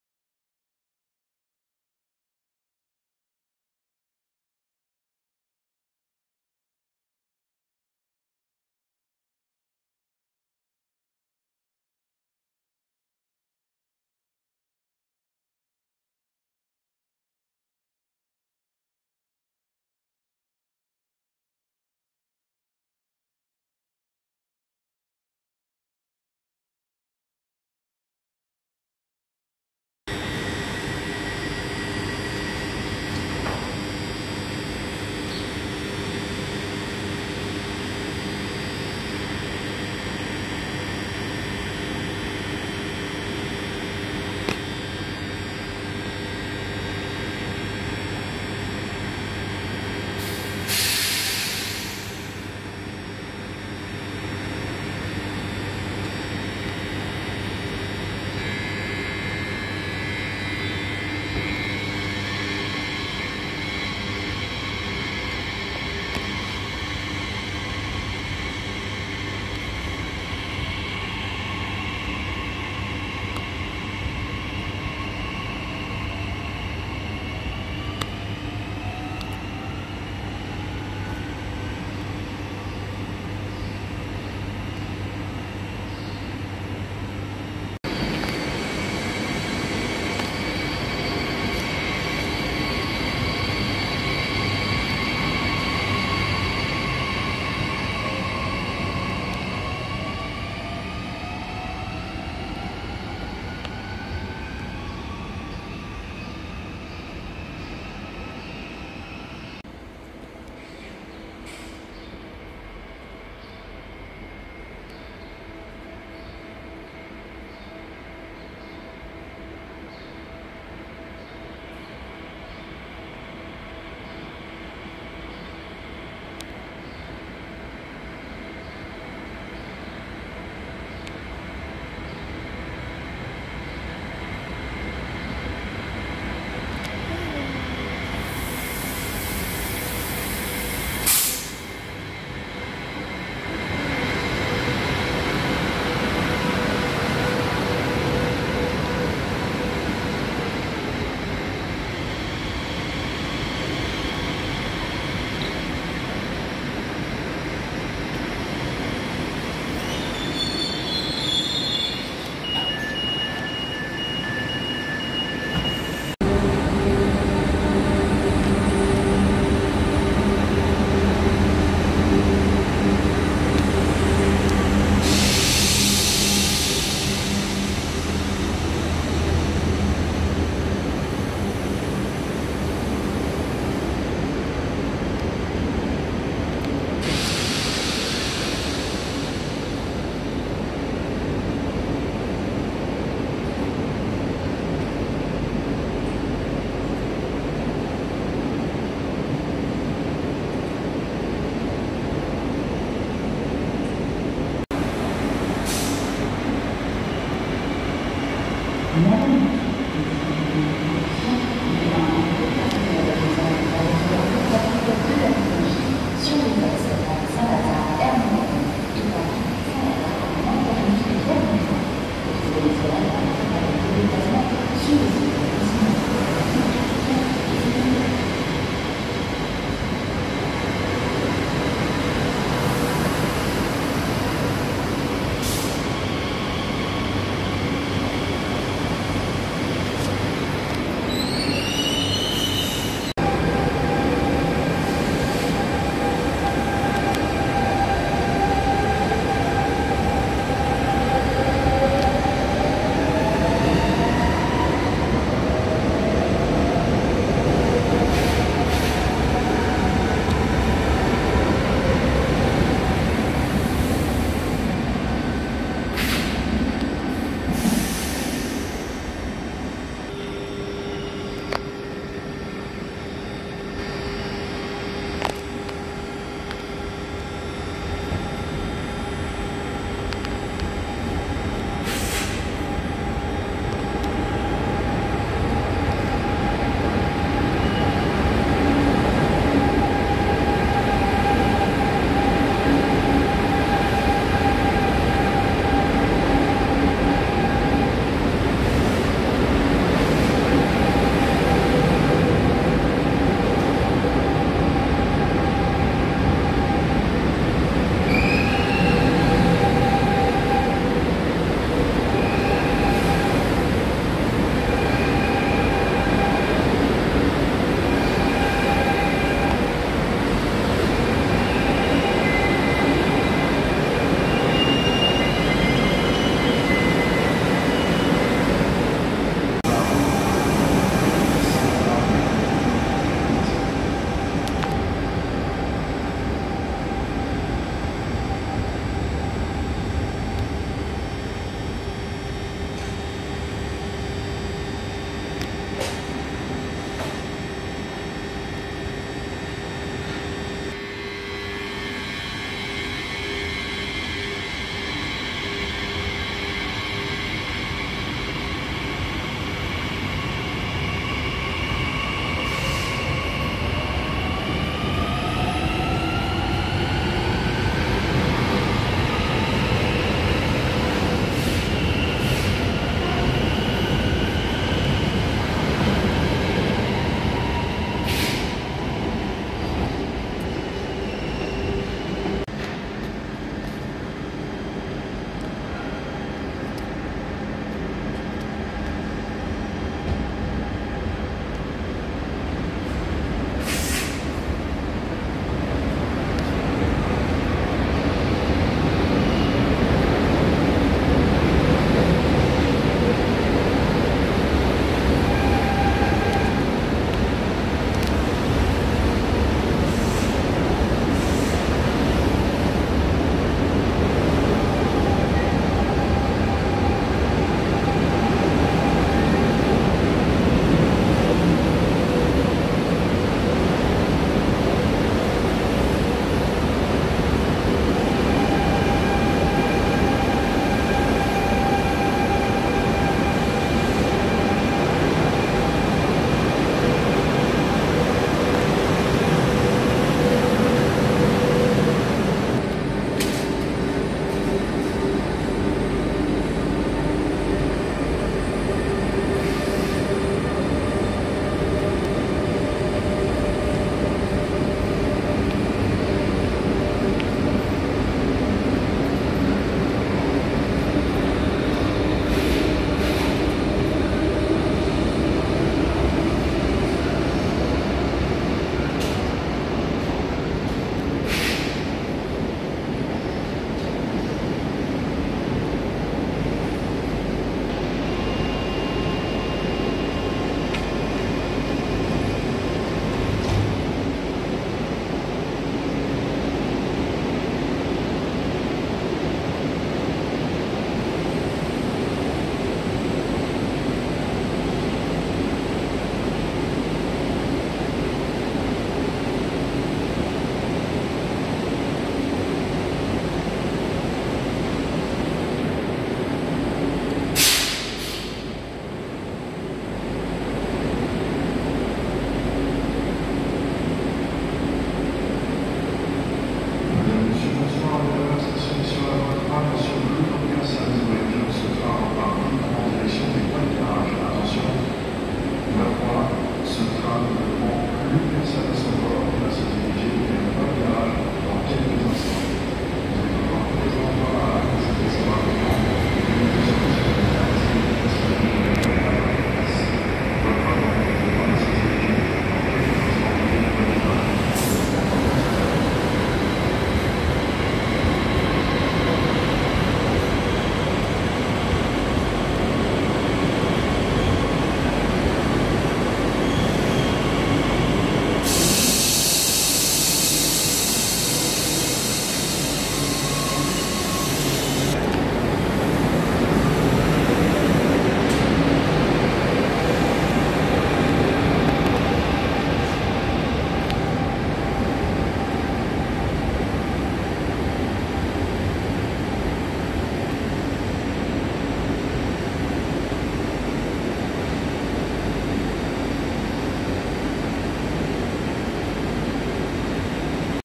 Spot en gare de Cergy le Haut
RER A: MI09 et MI2N
Transilien L: "NAT" Z50000